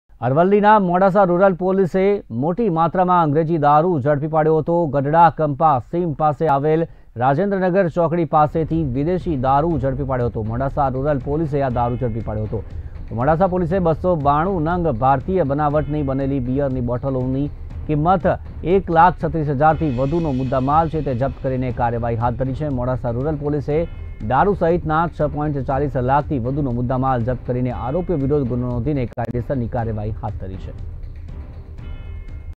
અરલલ્લી: મોડાસા રૂરલ પોલીસે મોટી માત્રામાં ઝડપ્યો વિદેશી દારૂ, 6.40 લાખથી વધુનો મુદ્દામાલ કર્યો જપ્ત
મોડાસા રૂરલ પોલીસે  વિદેશી દારૂ ઝડપી પાડ્યો
રાજેન્દ્રનગર ચોકડી પાસેથી વિદેશી દારૂ ઝડપી પાડ્યો
ભારતીય બનાવટની 912 બોટલ બીયર જપ્ત
પોલીસે કુલ 6.40 લાખથી વધુનો મુદ્દામાલ જપ્ત કર્યો
પોલીસે ગુનો નોંધી વધુ કાર્યવાહી હાથ ધરી